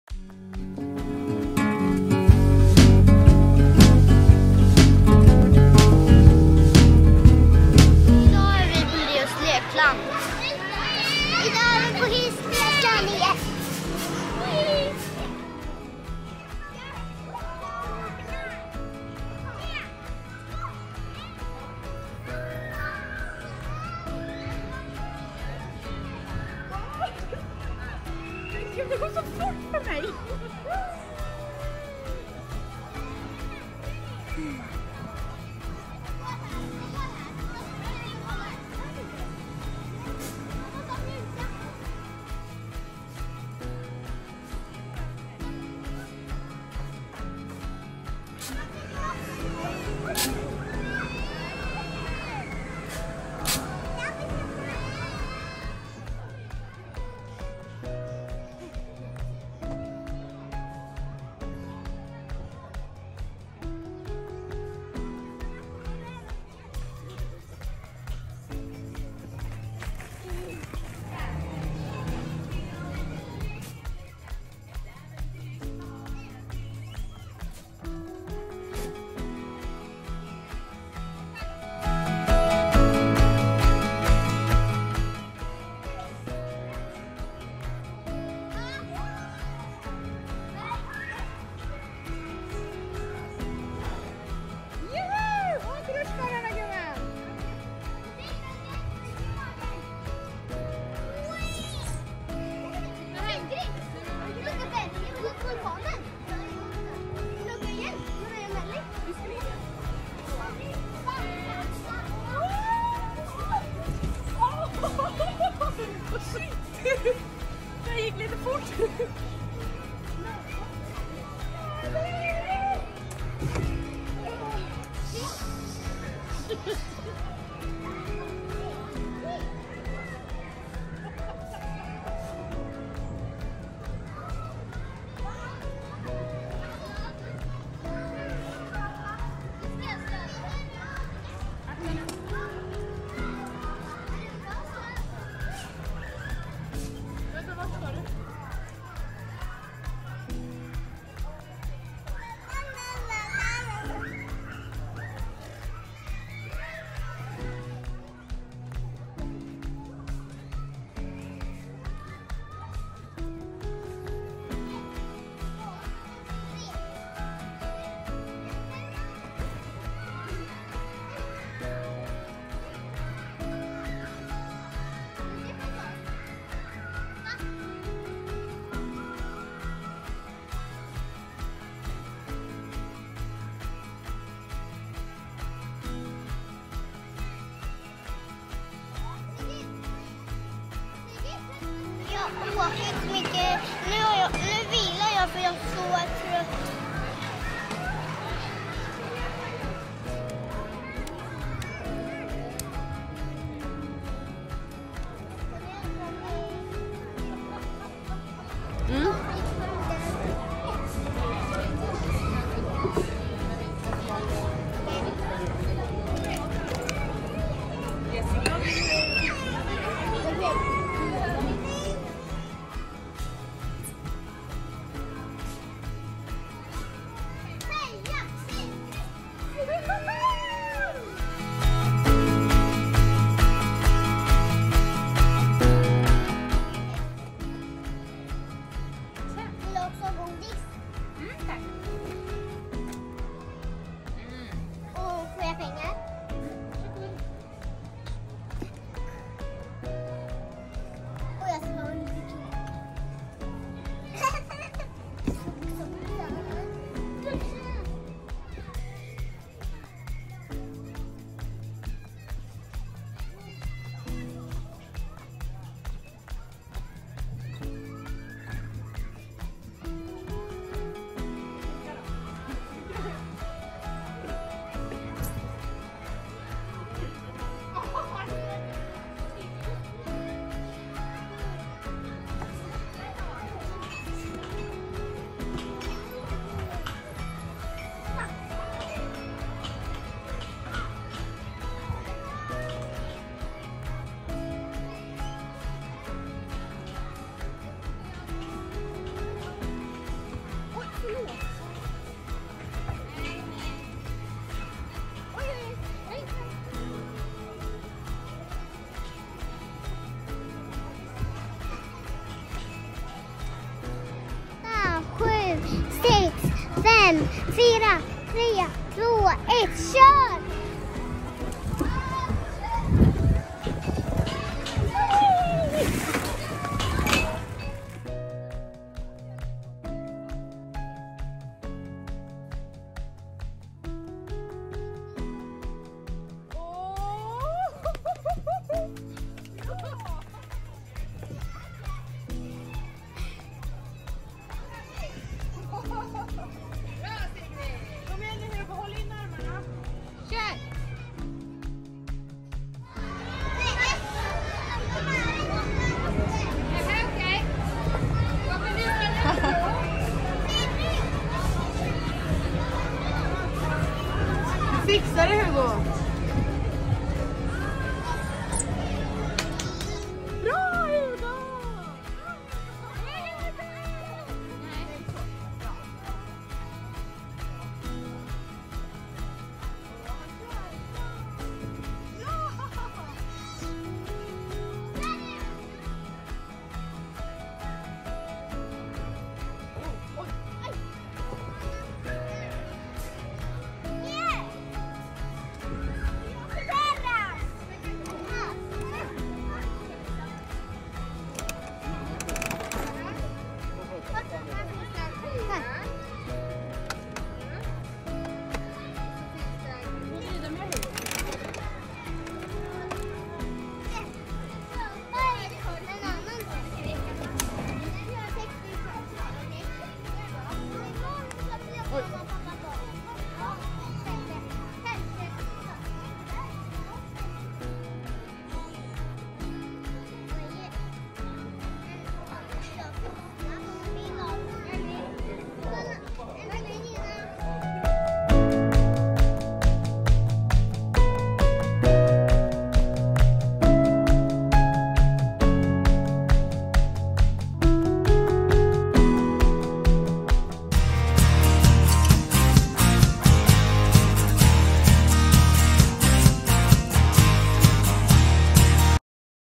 Leos Lekland i Karlstad. VLOGG
Vi besöker Leos Lekland i Karlstad tillsammans med storasyster Tilda och kompisen Nelly.